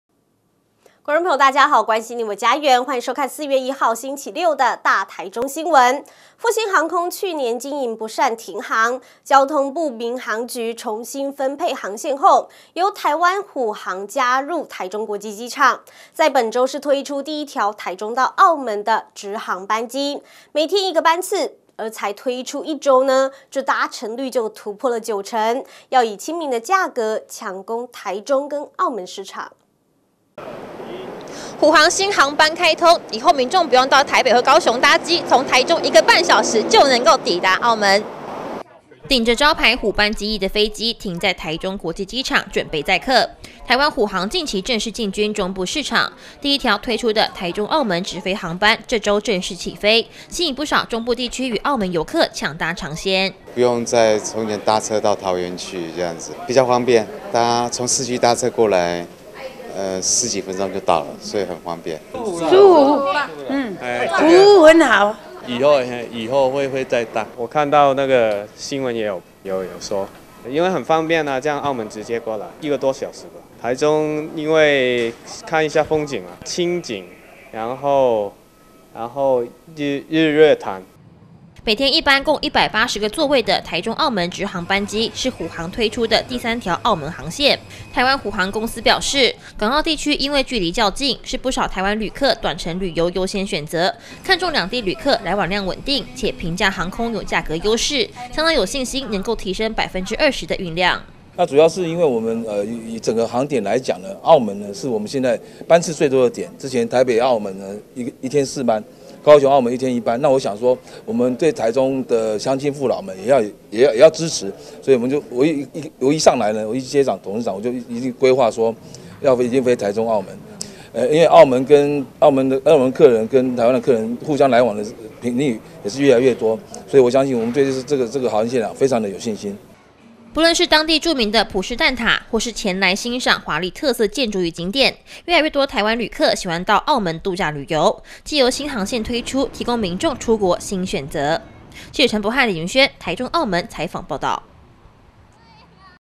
大台中新聞-台中-澳門新航線啟航 搭乘率破九成
復興航空去年經營不善停航，交通部民航局，重新分配航線後，由台灣虎航加入台中國際機場，在本週推出第一條的台中-澳門直航班機，每天一班次，才推出一週搭乘率突破九成，希望以較親民的價格，搶攻台中與澳門市場。「以上新聞可從哈TV APP收視」